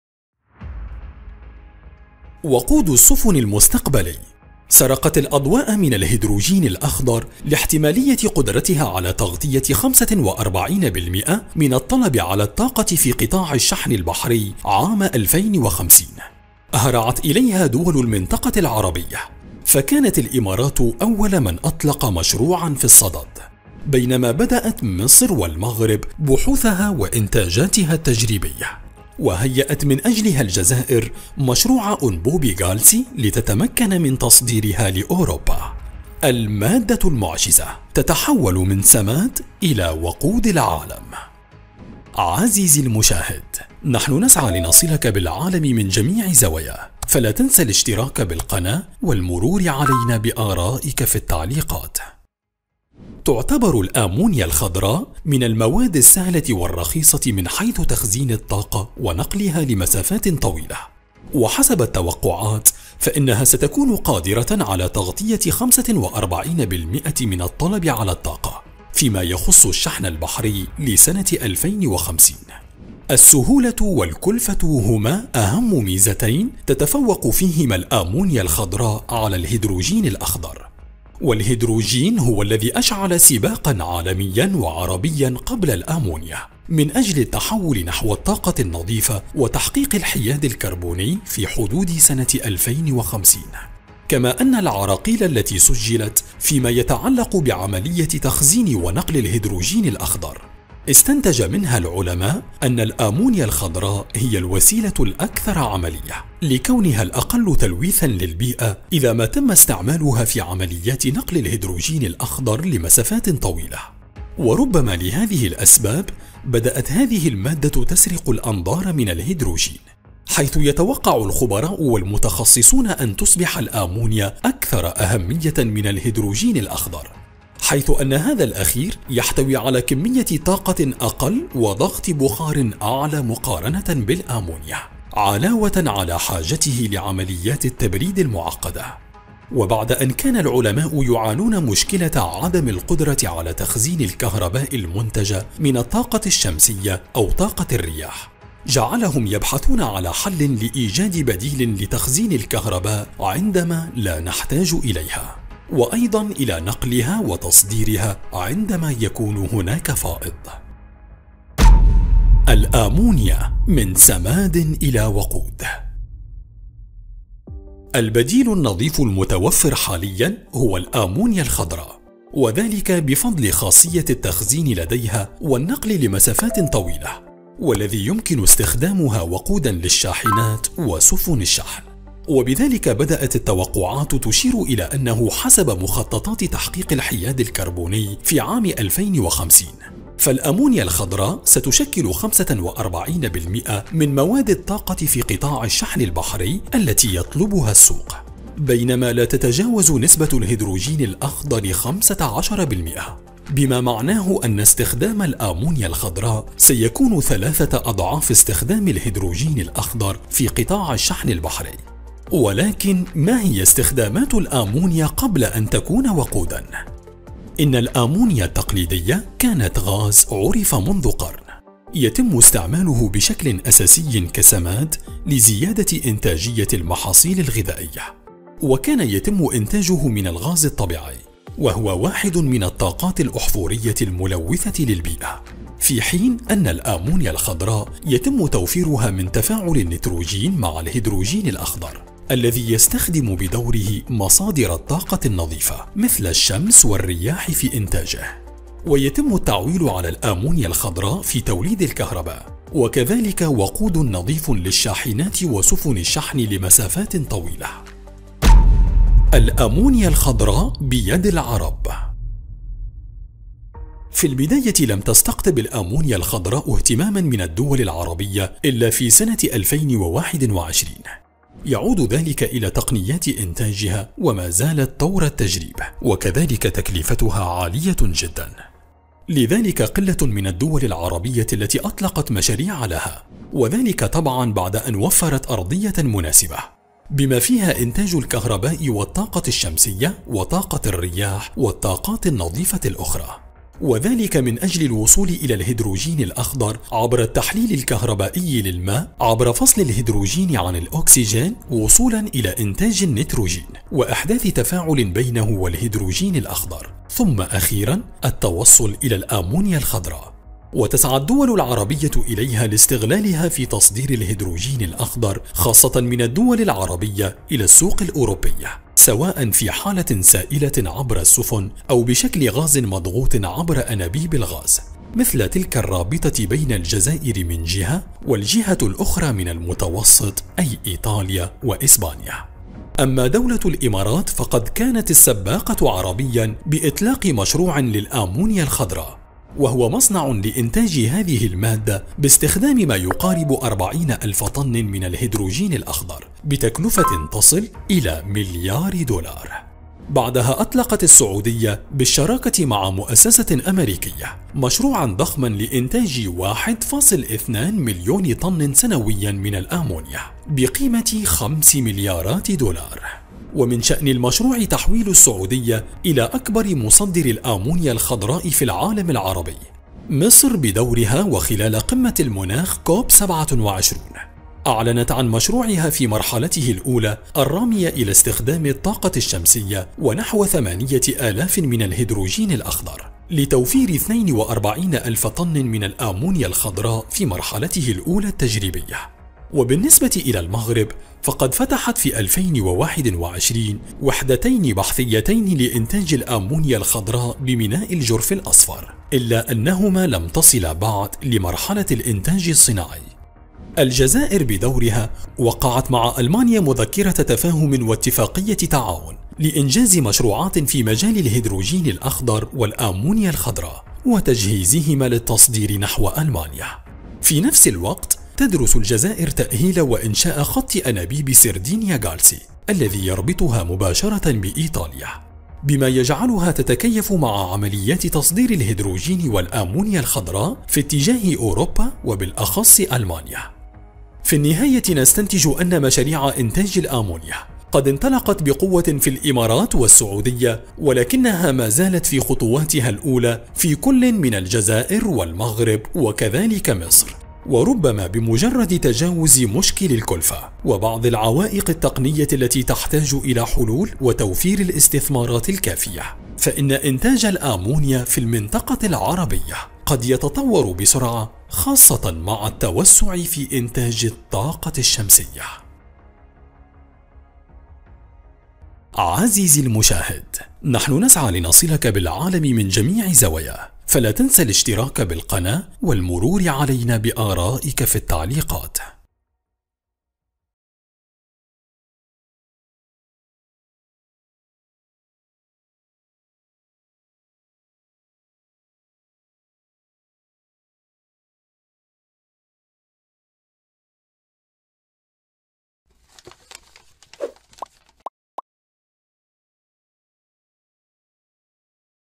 وقود السفن المستقبلي .. الأمونيا الخضراء ترأس مشاريع الجزائر المغرب والإمارات
نحدثكم في هذا الفيديو عن الأمونيا الخضراء المادة التي ستتحكم في مشاريع الطاقة النظيفة وتتحول إلى وقود السفن المستقبلي .. مشاريع بدأتها الإمارات والسعودية وتسير كل من الجزائر والمغرب مصر على خطاها فإليك التفاصيل

◀توضيح هام :
00:53 الأمونيا الخضراء
02:42 من سماد إلى وقود
04:19 الأمونيا الخضراء بيد العرب
06:43 شراكة الجزائر وألمانيا